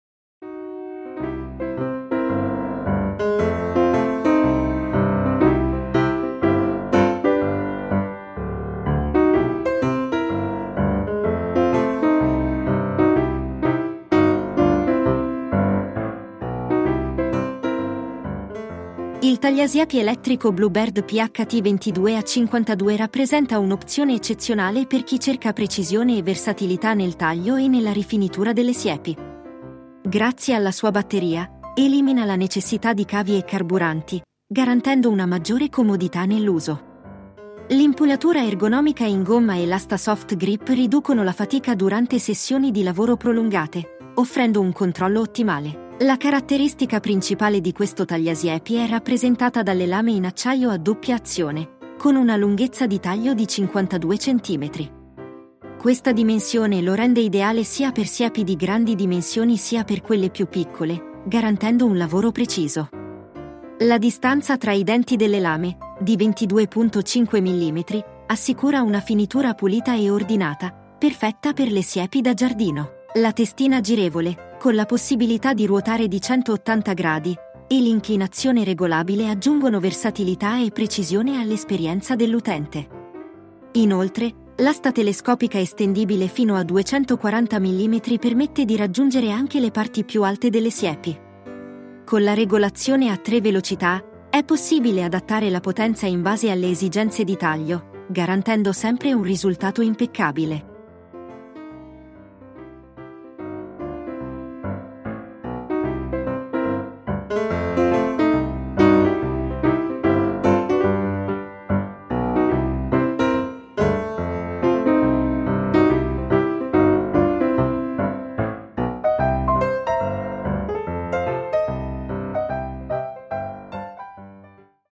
Presentazione del Blue Bird PHT 22-52 - Tagliasiepi su asta di prolunga a batteria - 21 V 5 Ah
*Ecco un video di presentazione del Blue Bird PHT 22-52 - Tagliasiepi su asta di prolunga a batteria - 21 V 5 Ah* *realizzato direttamente dal produttore per mostrarti tutte le principali caratteristiche.*

0:00-0:05 Intro
0:05-0:11 360° del prodotto
0:11-0:19 Unboxing
0:19-1:46 La macchina
1:46-1:51 360° del prodotto
1:51-1:57 Ordine ricambi
1:57-2:04 Outro

Il tagliasiepi elettrico Blue Bird PHT 22-52 è progettato per il taglio e la rifinitura di siepi di ogni dimensione, eliminando la necessità di cavi e carburanti, grazie alla sua batteria.

Macchina realizzata con motore brushless che garantisce prestazioni elevate e durature nel tempo.

L'impugnatura di gomma ergonomica e parte dell'asta con rifinitura soft grip assicurano un uso agevole e riducono la fatica anche durante sessioni di lavoro prolungate.

La caratteristica principale del tagliasiepi Blue Bird PHT 22-52 sono le sue lame in acciaio a doppia azione, che offrono una lunghezza di taglio di 52 cm. Questa dimensione lo rende adatto sia per siepi di grandi dimensioni sia per quelle più piccole, garantendo un lavoro di precisione.

La distanza tra i denti delle lame, di 22.5 mm, è ottimizzata per una finitura pulita e ordinata, ideale per le tipiche siepi da giardino.

Un'altra caratteristica distintiva di questo modello è la testina girevole, che può ruotare di gradi, permettendo così all'utente di personalizzare e rifinire le piante in modo sicuro e confortevole. La regolazione dell'inclinazione, fino a 180°, aggiunge un ulteriore livello di versatilità e precisione.

Inoltre, l'asta telescopica, estendibile fino a 240mm, rende questo tagliasiepi adatto per raggiungere anche le parti più alte delle siepi.

La regolazione a 3 velocità consente all'utente di adattare la potenza in base alle diverse esigenze di taglio, assicurando così un controllo ottimale durante l'uso.